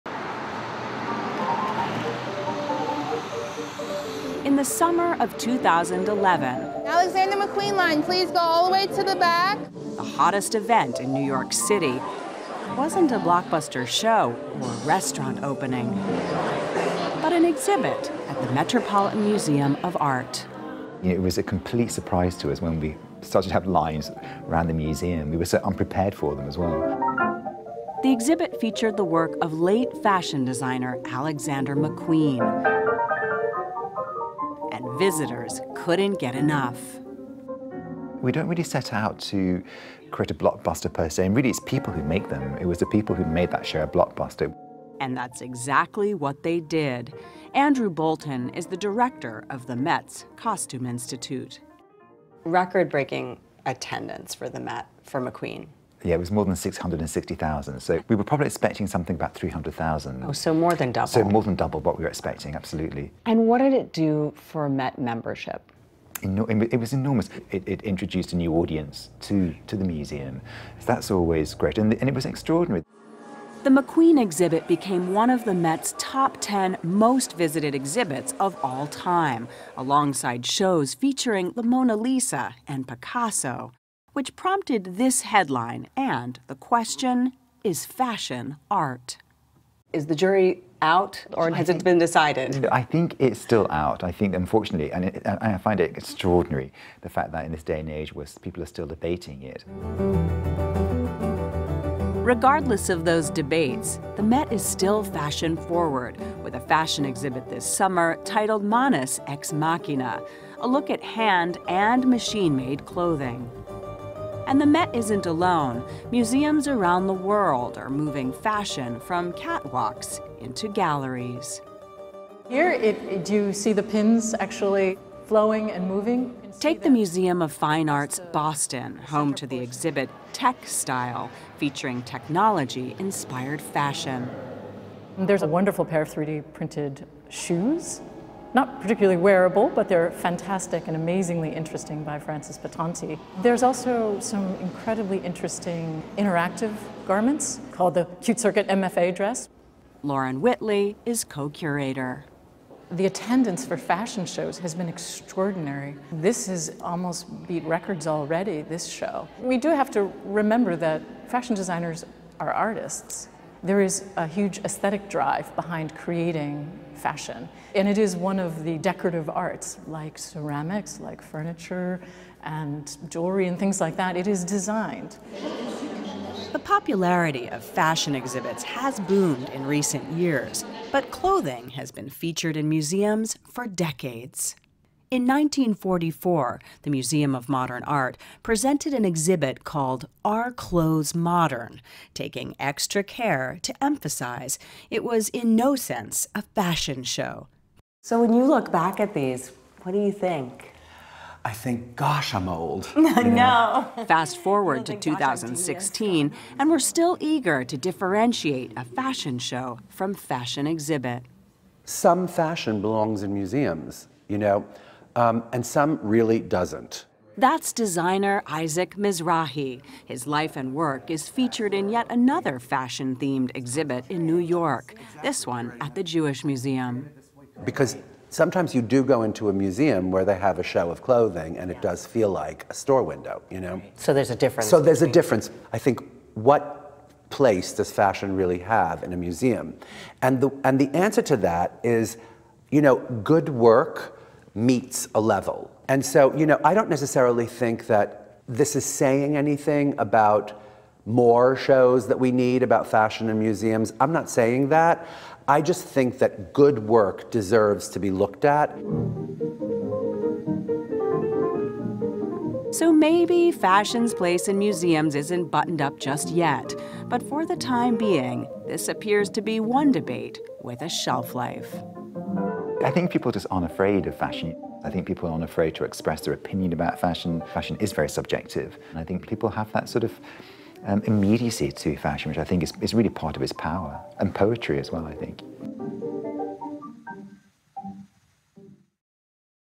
Fashion collections turn museums into runway for past designs
During award season, dresses donned by stars of the entertainment world are occasionally praised as works of art. Similar to Picasso and Matisse, clothing designers are getting the red carpet treatment in some of America's well known museums. Serena Altschul reports.